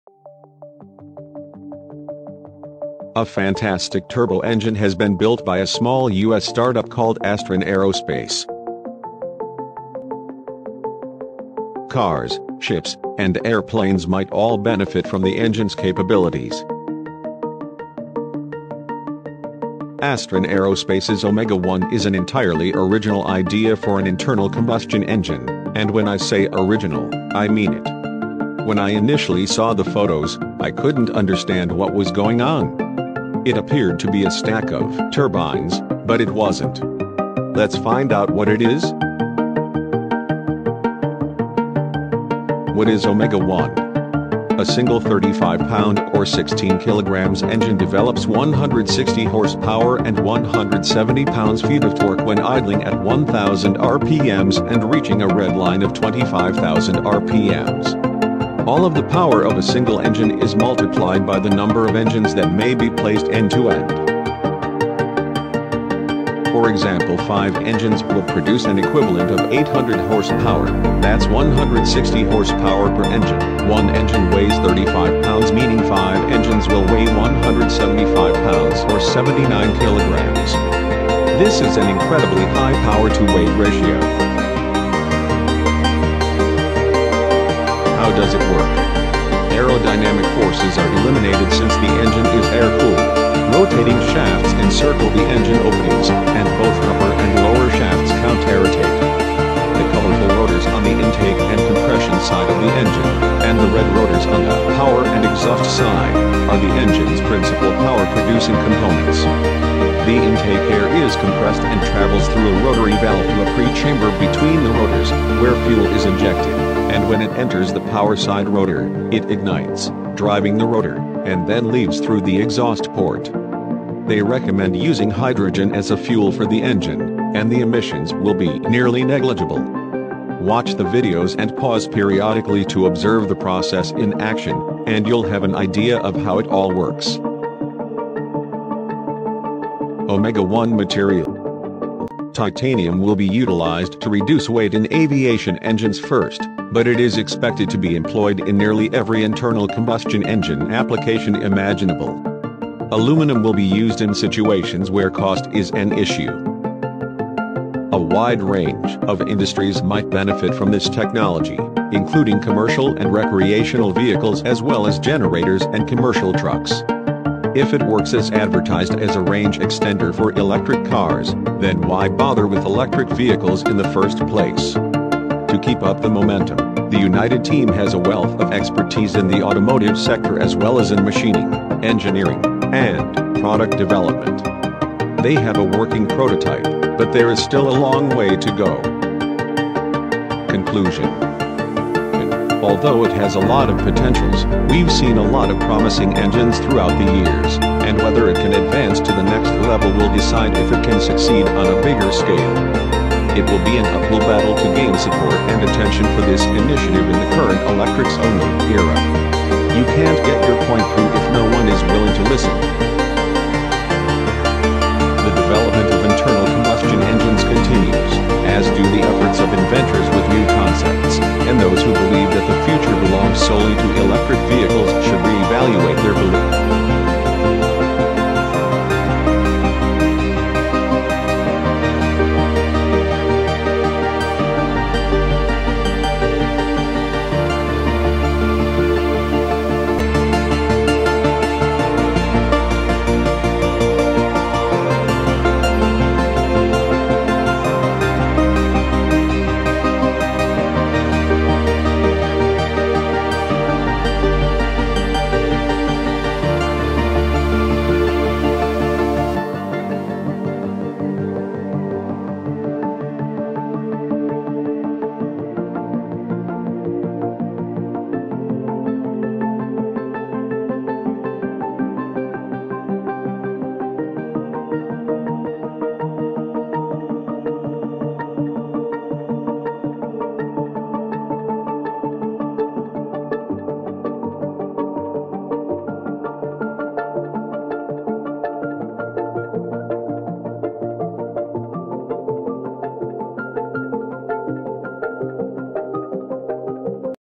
Omega 1: The Most Revolutionary Engine In The World - The Piston-Less near Zero Emissions Engine
A fantastic turbo engine has been built by a small US startup called Astron Aerospace.  Cars, ships, and airplanes might all benefit from the engine's capabilities.

Astron Aerospace's Omega 1 is an entirely original idea for an internal combustion engine, and when I say original, I mean it. When I initially saw the photos, I couldn't understand what was going on. It appeared to be a stack of turbines, but it wasn't. Let's find out what it is?

What is Omega 1? -
A single 35-pound or 16 kg engine develops 160 hp and 170 lb feet of torque when idling at 1000 rpm and reaching a redline of 25,000 rpm. All of the power of a single-engine is multiplied by the number of engines that may be placed end-to-end.

For example 5 engines will produce an equivalent of 800 hp, that's 160 hp per engine.
One engine weighs 35 lb meaning 5 engines will weigh 175 pounds or 79 kg. This is an Incredibly high power to weight ratio.

Aerodynamic forces are eliminated since the engine is air-cooled, rotating shafts encircle the engine openings, and both upper and lower shafts counterrotate.

The colorful rotors on the intake and compression side of the engine, and the red rotors on the power and exhaust side, are the engine's principal power-producing components.

The intake air is compressed and travels through a rotary valve to a pre-chamber between the rotors, where fuel is injected, and when it enters the power side rotor, it ignites, driving the rotor, and then leaves through the exhaust port. They recommend using hydrogen as a fuel for the engine, and the emissions will be nearly negligible.

Watch the videos and pause periodically to observe the process in action, and you'll have an idea of how it all works.

Omega 1 Material -
Titanium will be utilized to reduce weight in aviation engines first, but it is expected to be employed in nearly every internal combustion engine application imaginable. Aluminum will be used in situations where cost is an issue.

A wide range of industries might benefit from this technology, including commercial and recreational vehicles as well as generators and commercial trucks. If it works as advertised as a range extender for electric cars, then why bother with electric vehicles in the first place?

To keep up the momentum, the united team has a wealth of expertise in the automotive sector as well as in machining, engineering, and product development. They have a working prototype, but there is still a long way to go.

Conclusion -

Although it has a lot of potentials, we've seen a lot of promising engines throughout the years, and whether it can advance to the next level will decide if it can succeed on a bigger scale. It will be an uphill battle to gain support and attention for this initiative in the current electrics-only era. You can't get your point through if no one is willing to listen.

The development of internal combustion engines continues, as do the efforts of inventors with new concepts, and those who believe that the future belongs solely to electric vehicles should reevaluate their beliefs.